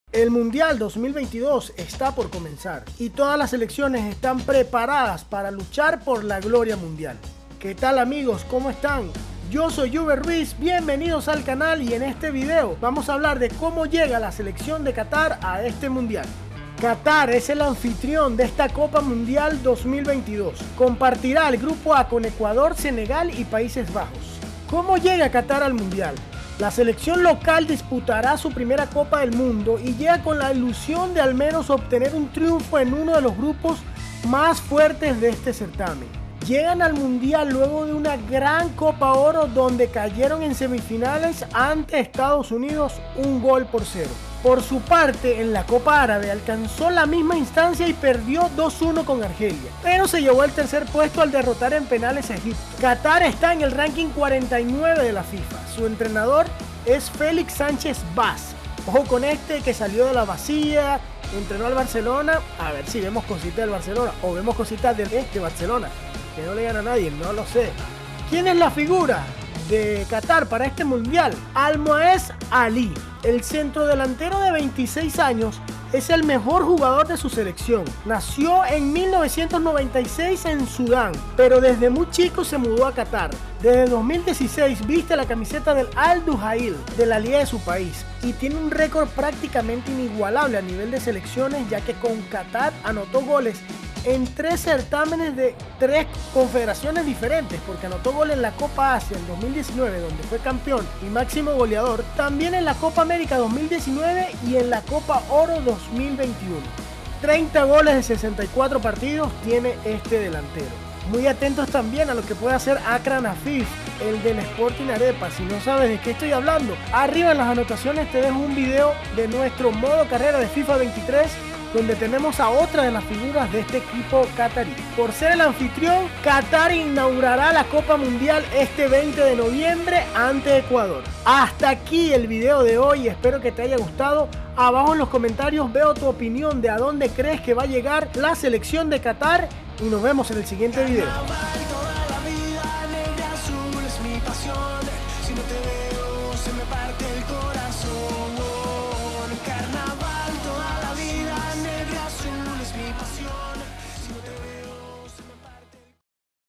¿COMO LLEGA QATAR🇶🇦 AL MUNDIAL? | La Guía del Mundial Qatar 2022⚽️🏆🌎
El Mundial Qatar 2022 ya tiene todo listo. Mientras millones de fanáticos alrededor del planeta ya empiezan a soñar con los partidos y pensar en cómo podría ser el camino de su equipo al máximo torneo futbolístico del mundo, es buen momento para una guía de cada equipo participante. En el este video: Qatar.